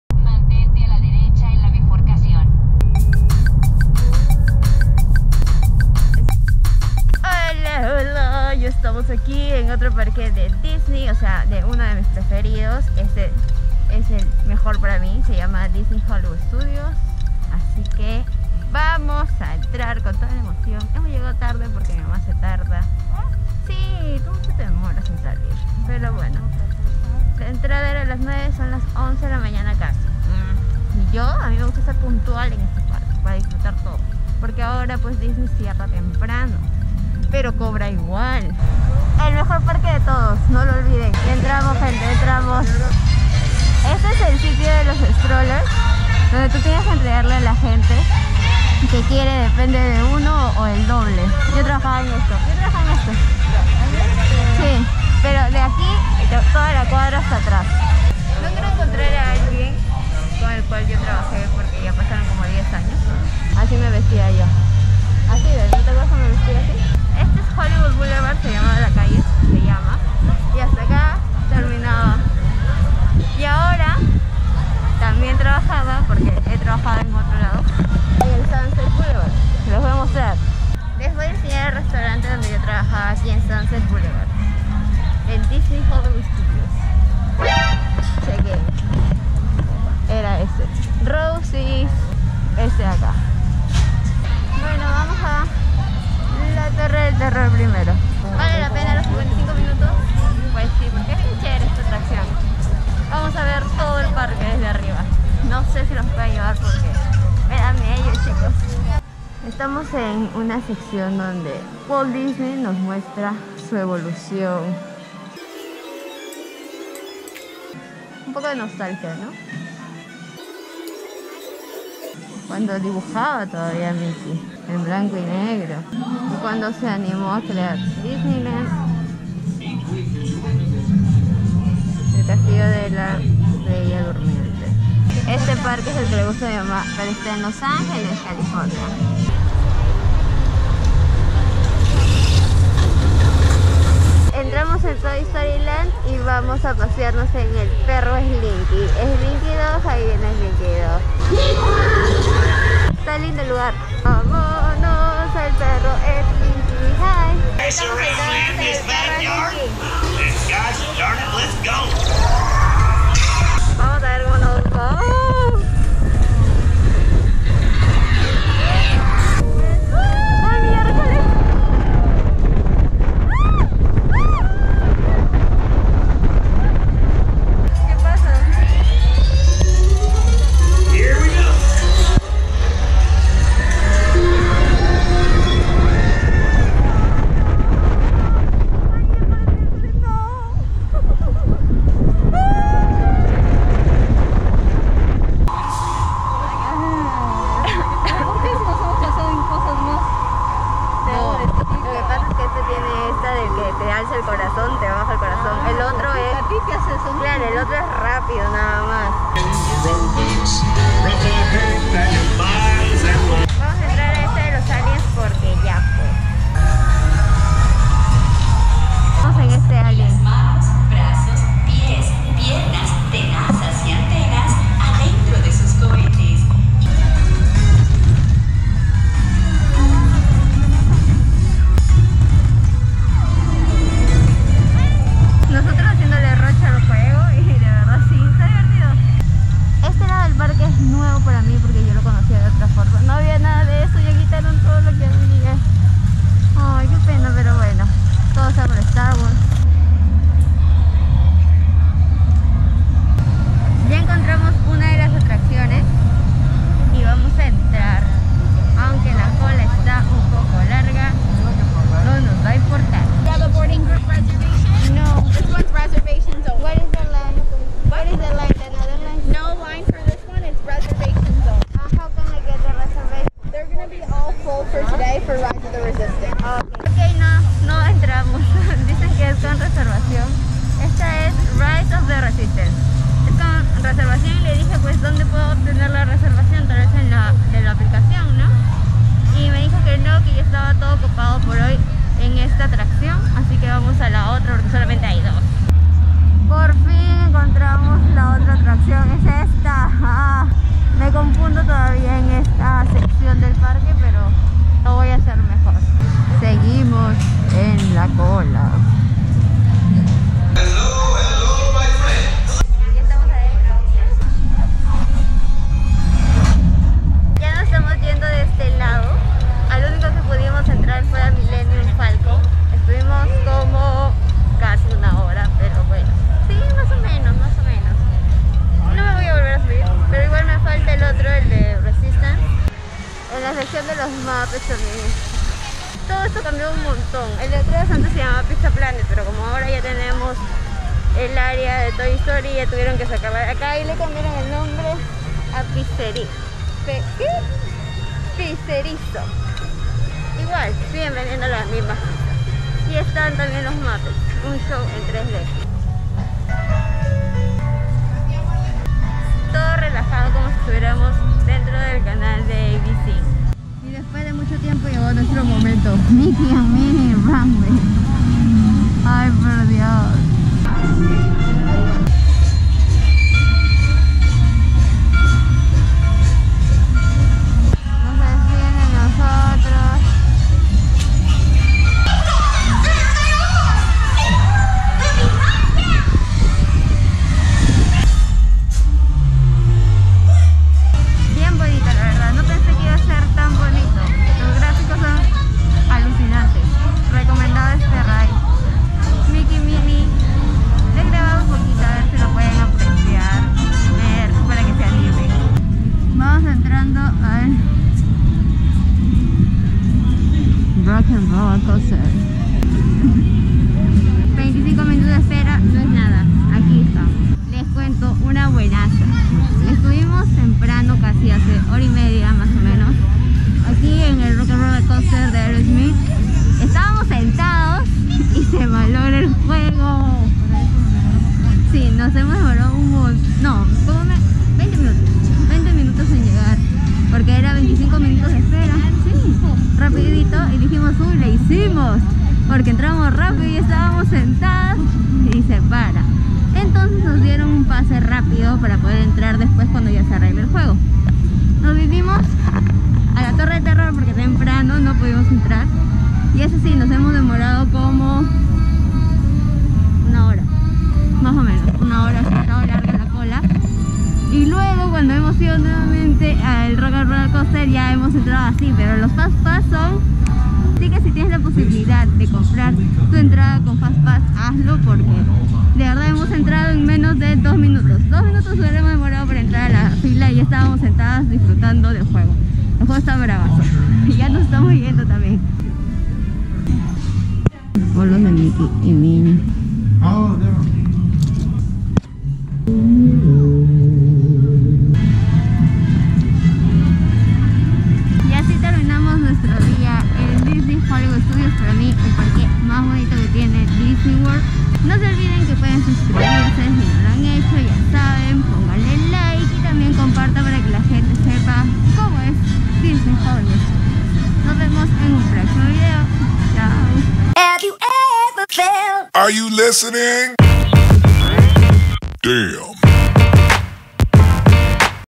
Trabajé en DISNEY HOLLYWOOD STUDIOS y les enseño donde mientras paseo en el parque
🌈 Encuéntrame en:

💛 TIKTOK  tefagonzalezc